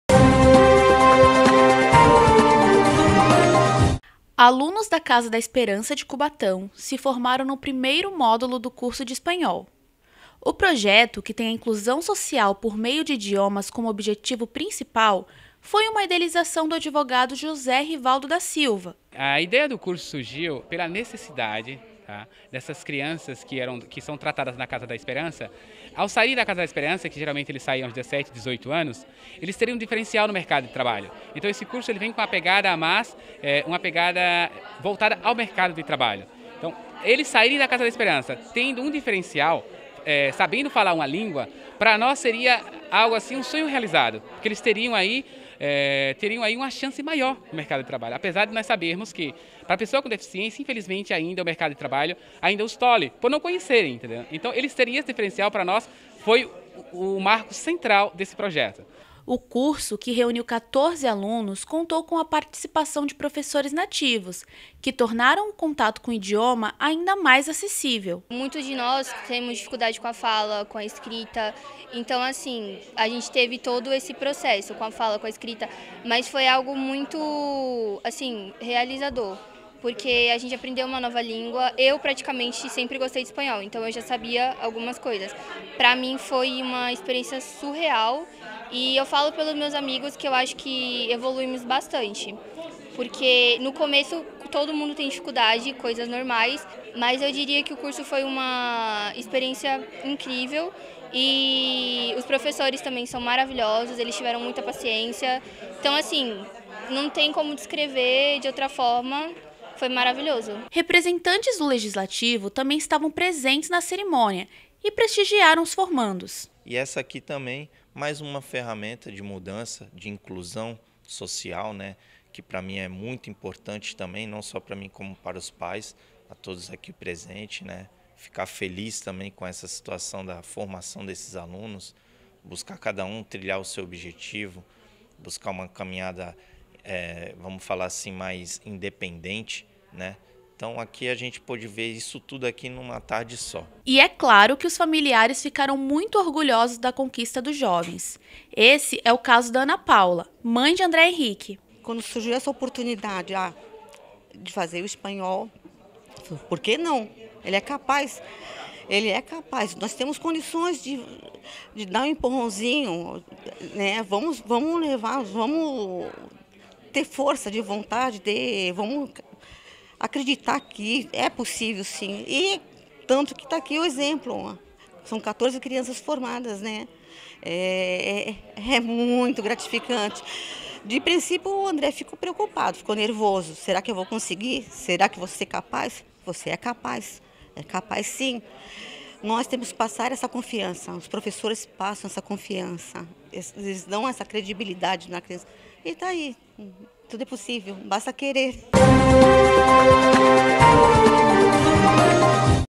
ALUNOS DA CASA DA ESPERANÇA DE CUBATÃO SE FORMAM EM ESPANHOL
A colação faz parte do Módulo 1 do Proyecto El Spañol como Herramienta de Incusión Social